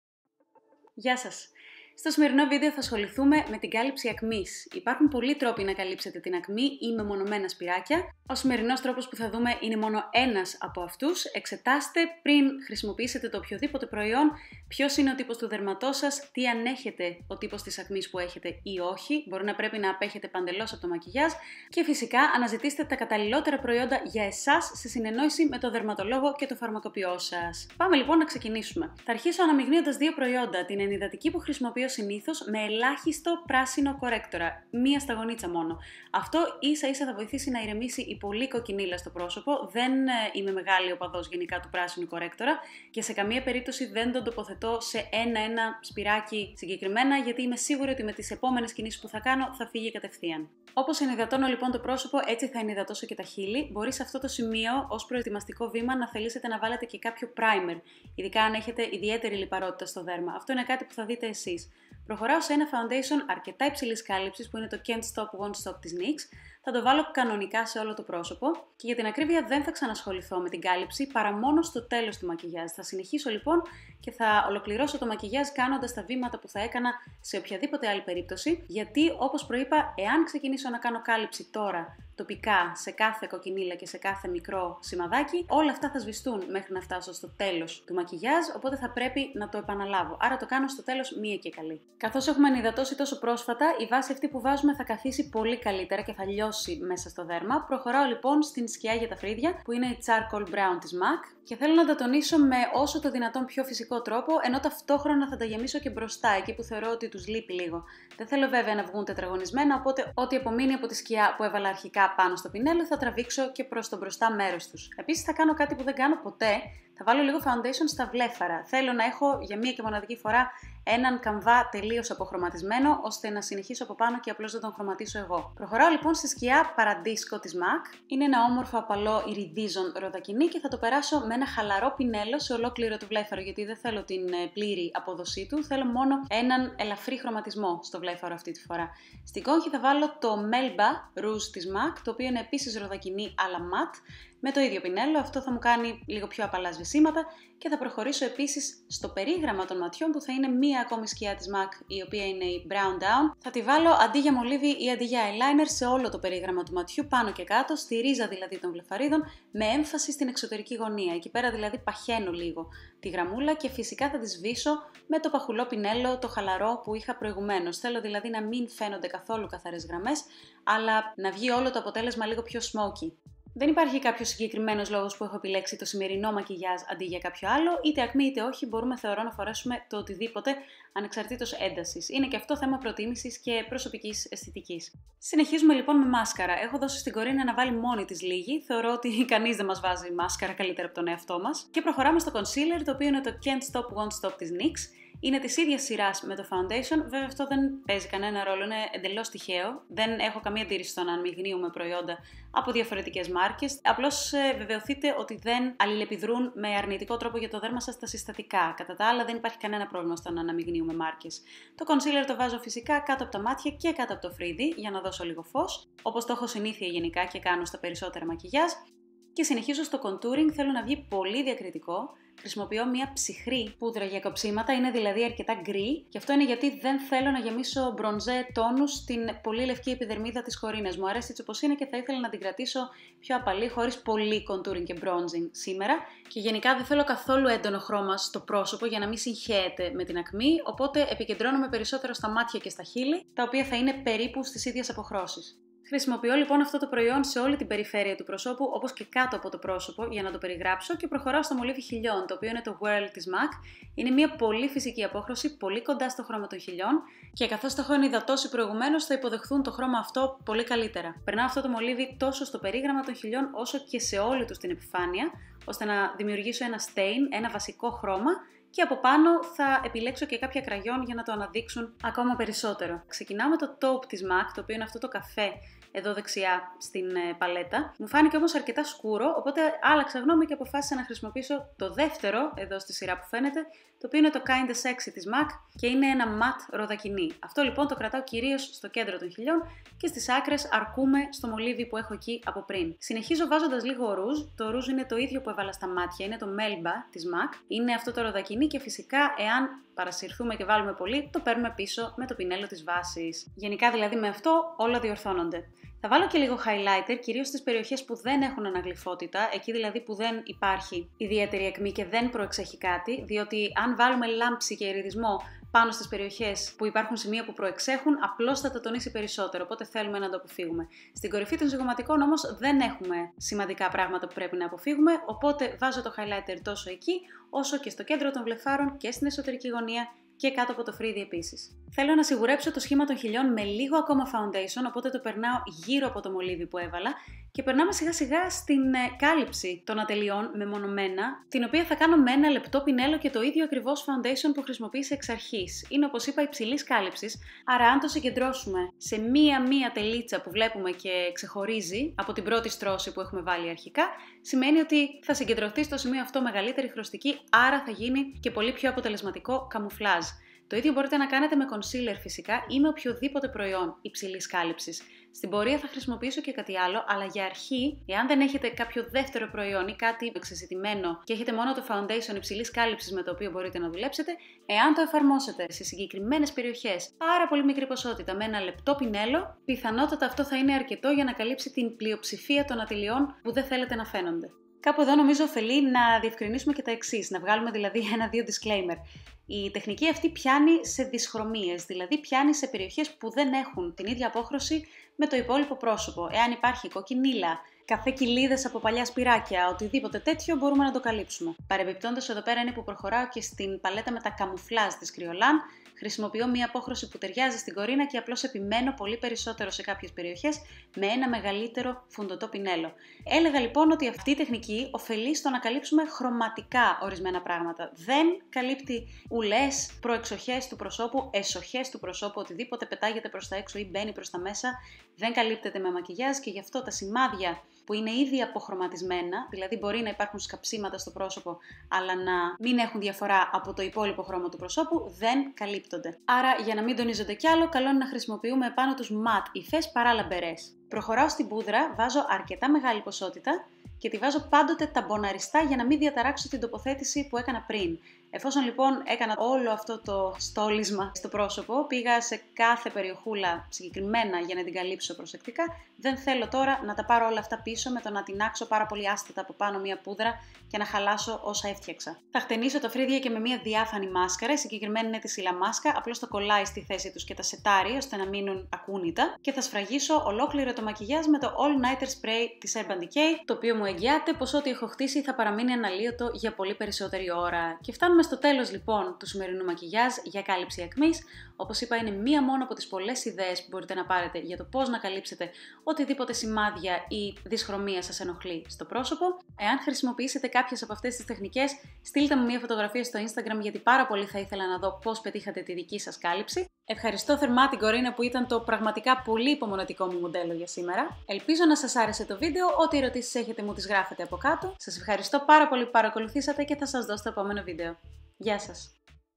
Πώς Να Καλύψετε Αποτελεσματικά Την Ακμή
Μία απ'τις πολλές μεθόδους που μπορείτε να χρησιμοποιήσετε για να καμουφλάρετε την ακμή.

Λίστα προϊόντων:

Emrbyolisse lait creme concentre
Inglot 36 cream concealer
Nuxe Reve De Miel lip treatment
Nyx Can't Stop Won't Stop foundation
Mac Charcoal Brown eyeshadow
Mac Paradisco eyeshadow
Mac Melba blush
Mac Brown Down eyeshadow
Maybelline Colossal mascara
Nyx Can't Stop Won't Stop concealer
Mac Taupe contour powder
Mac Whirl lip pencil
Mac Taupe lipstick
Mac Kinda Sexy lipstick
Mac Vanilla pigment
Kryolan Dermacolor palette
Kryolan dry powder
Illamasqua Brow & Lash gel

Το βίντεο δεν περιέχει τοποθέτηση προϊόντων.